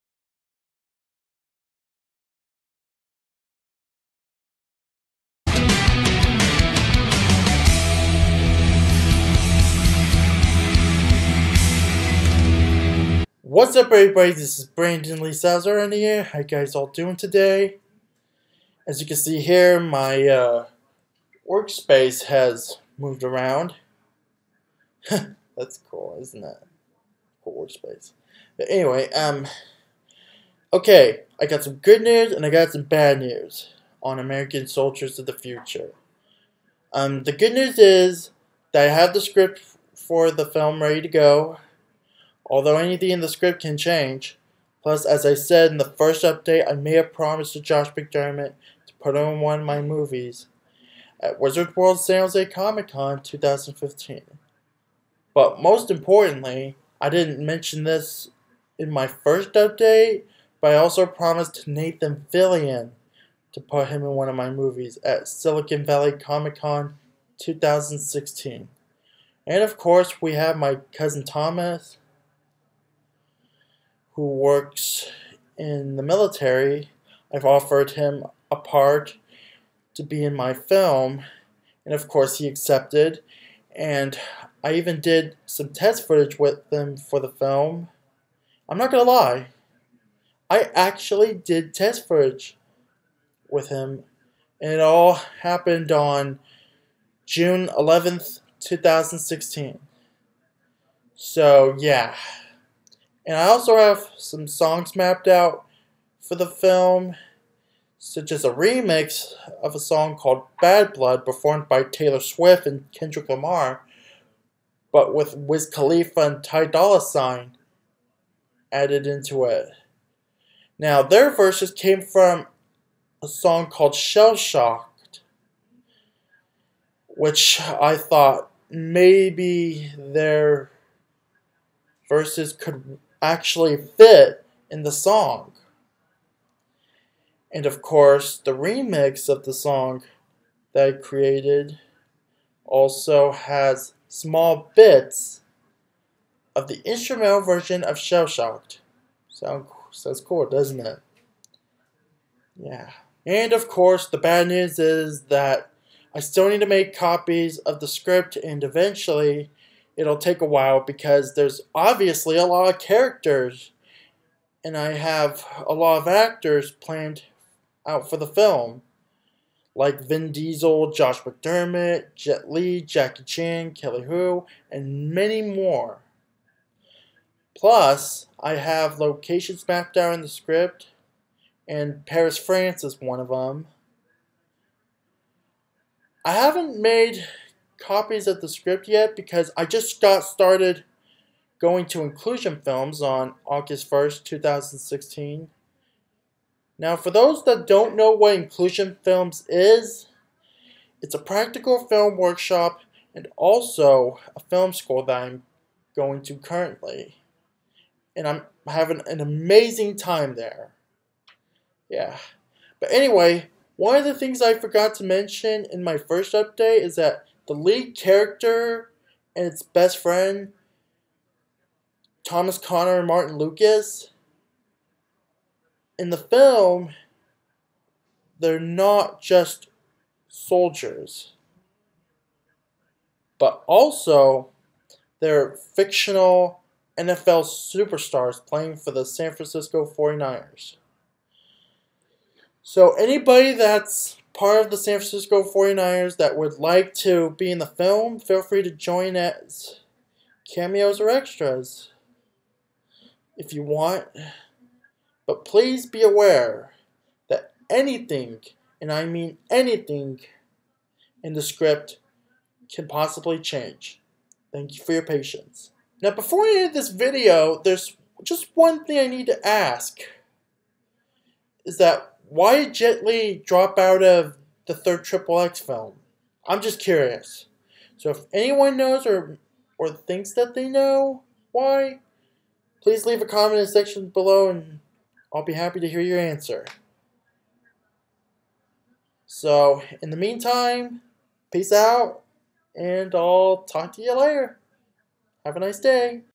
American Soldiers of the Future movie update # 5 + One Quick Question On The New xXx Movie
This is my 5th update on my new film "American Soldiers of the Future" plus one quick question on the new xXx movie.